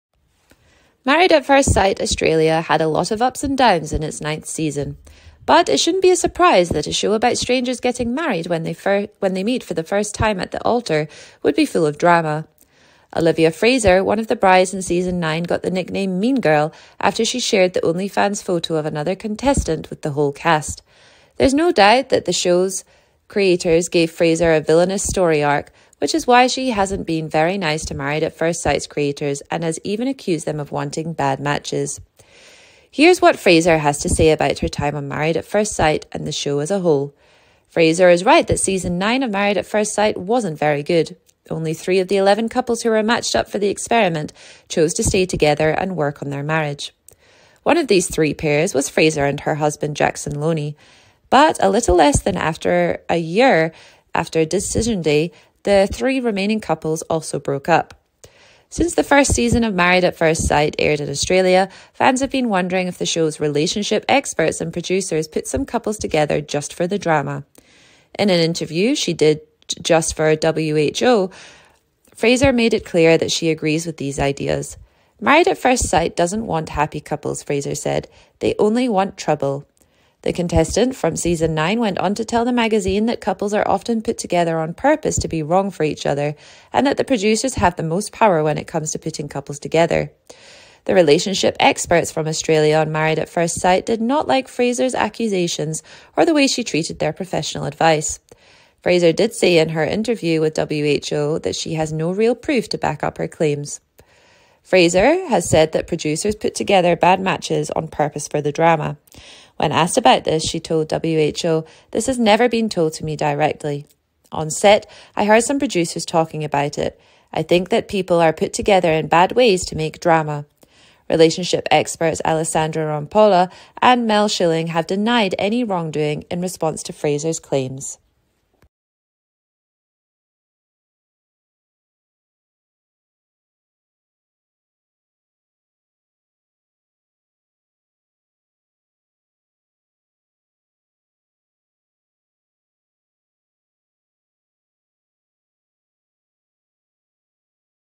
Married at First Sight: Contestant Exposes the Producers for wanting "Mismatches"
Since the first season of Married at First Sight aired in Australia, fans have been wondering if the show's "relationship experts" and producers put some couples together just for the drama.